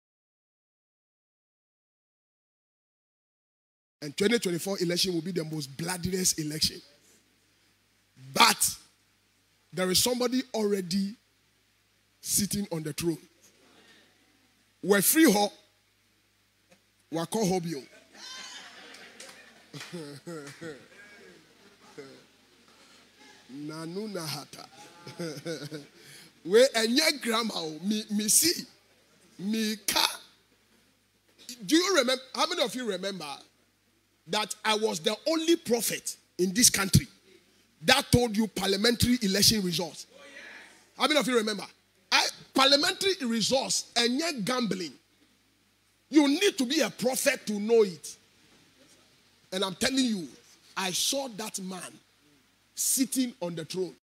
I see a former president of Ghana become president - Prophet Eric Parsley
Prophet Eric Parsley's prophecy about the eventual winner of the 2024 presidential polls in Ghana, West Africa.